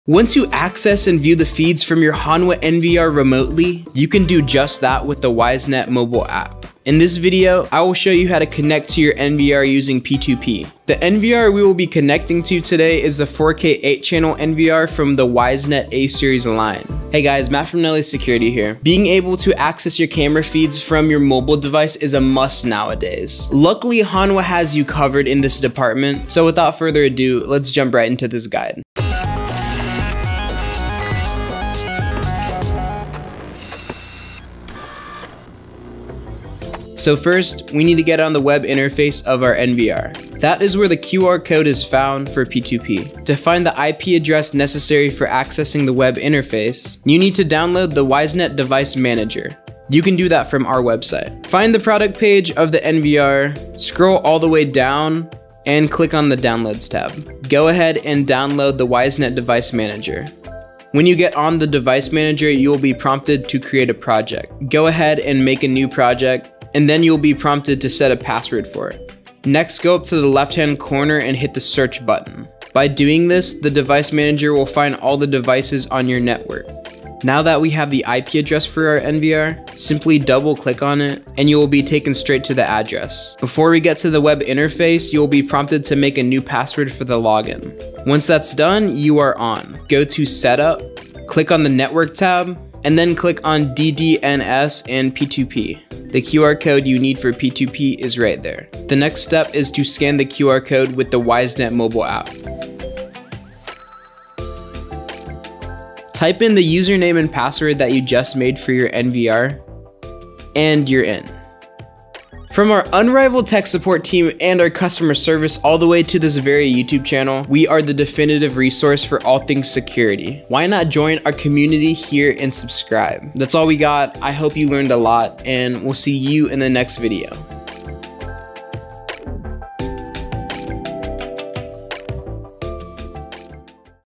How to Connect your Hanwha NVR to the Wisenet Mobile App with P2P + Finding IP Address
Want to access and view the feeds of your Hanwha NVR remotely? You can do that with the Wisenet mobile app! In this video, we will show you how to connect to your NVR using P2P. We will scan the QR code inside the NVR’s web interface using the app. On top of connecting the NVR to the mobile app, we also go over the process of finding IP address via the Wisenet Device Manager.

📋 Video Outline

00:00 - Introduction
00:37 - Finding IP address with Wisenet Device Manager
01:28 - Finding P2P in web interface
01:39 - Using Wisenet Mobile app to scan QR code
01:52 - Conclusion

🔗 Product Links